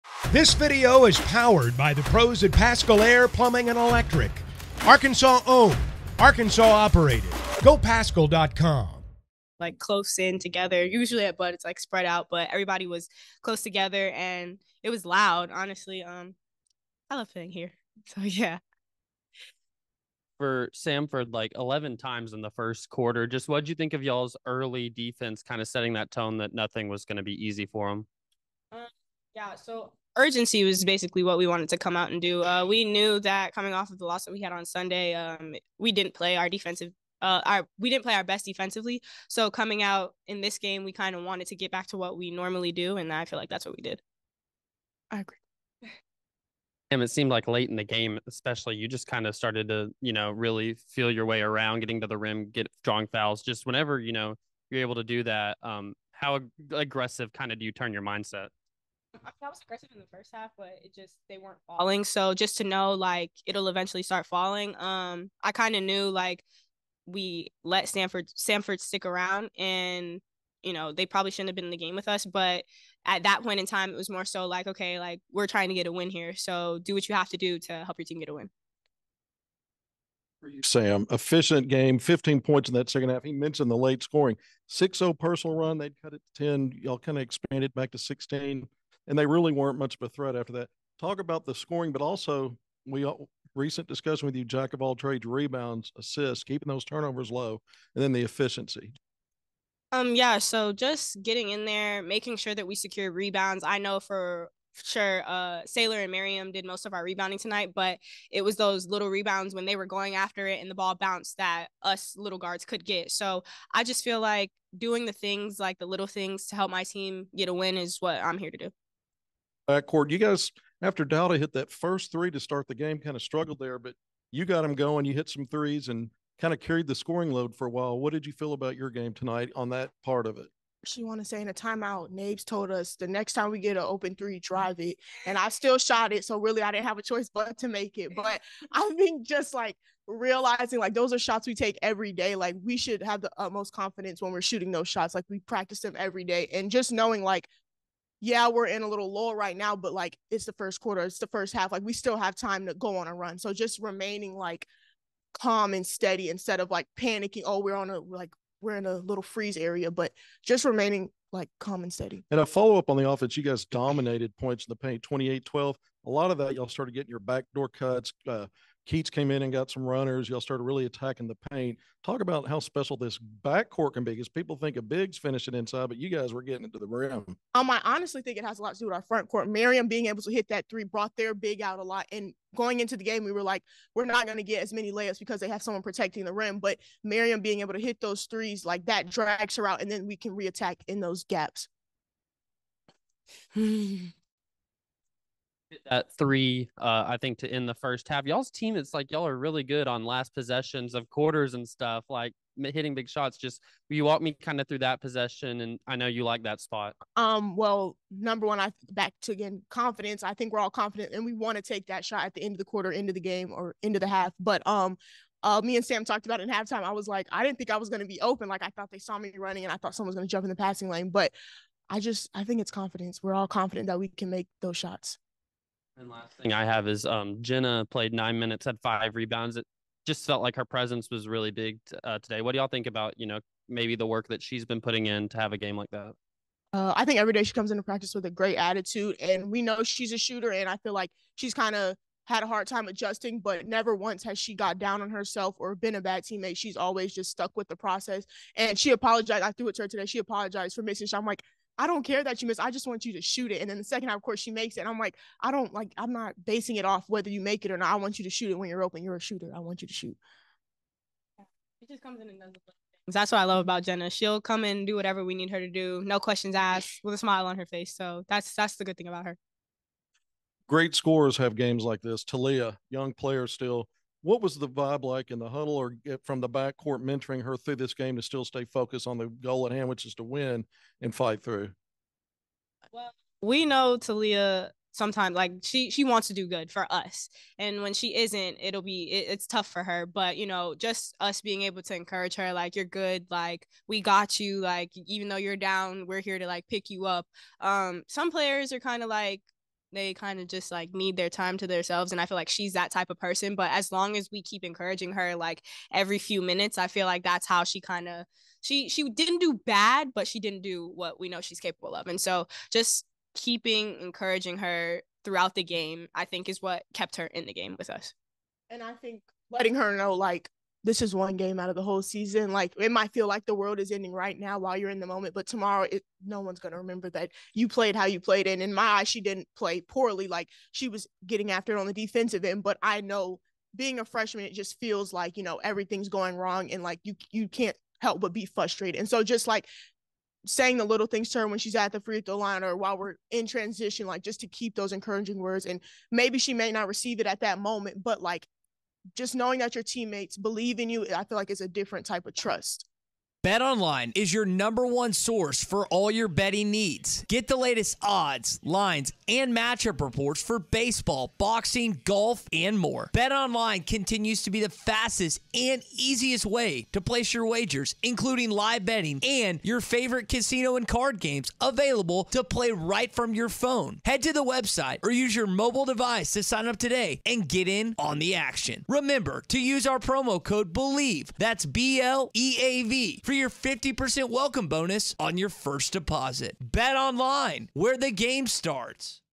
Razorbacks' Samara Spencer, Makayla Daniels on win over Samford
Overcoming struggles of first half with big second second half and some different people doing the scoring.